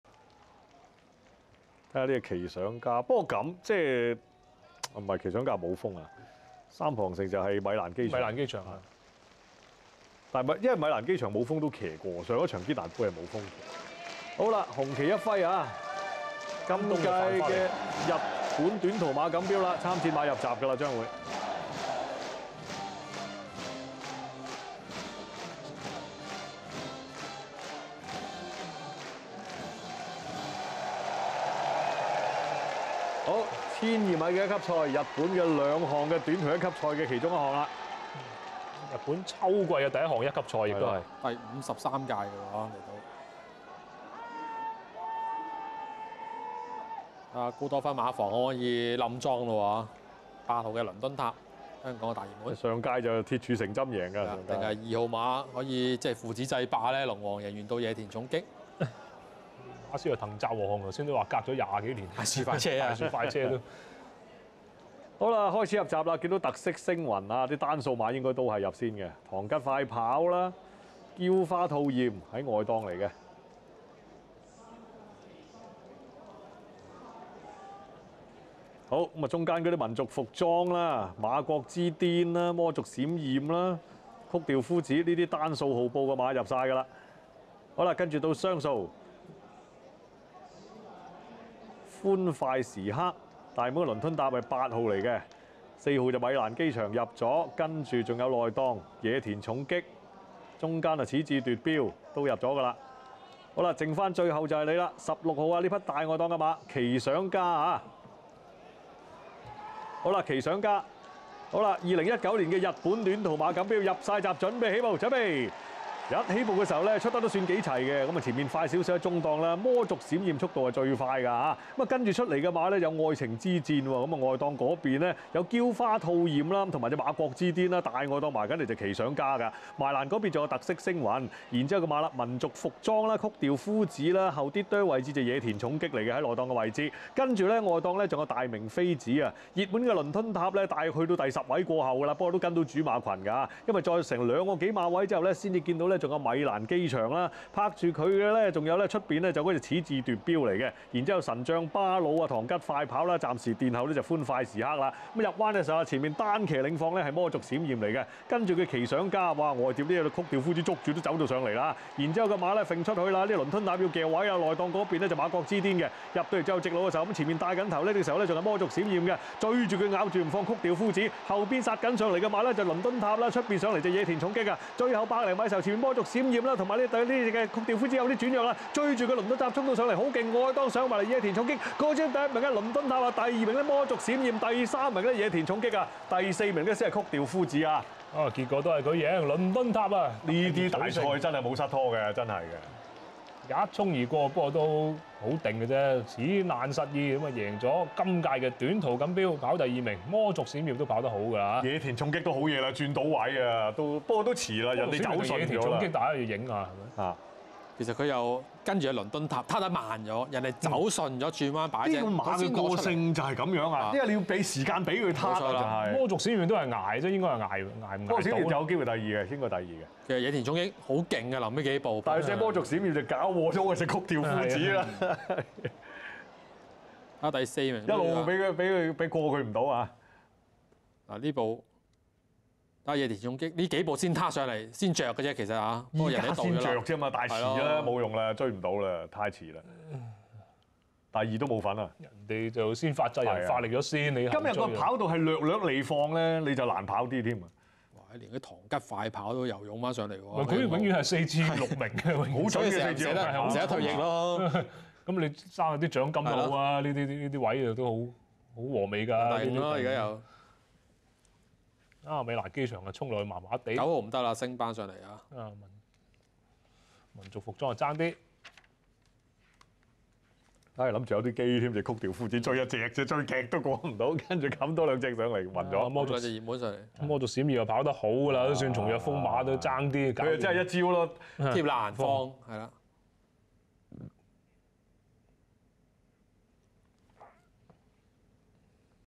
2019 日本短途馬錦標 Sprinters Stakes スプリンターズステークス 倫敦塔 Tower Of London タワーオブロンドン
29/09/2019
短途馬錦標　國際一級賽 1200M　草地
Sprinters Stakes (G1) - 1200M Turf

名次：
倫敦塔　(日)　　李慕華　　　藤澤和雄
魔族閃焰(美)　　松若風馬　　音無秀孝
野田重擊(日)　　川田將雅　　安田隆行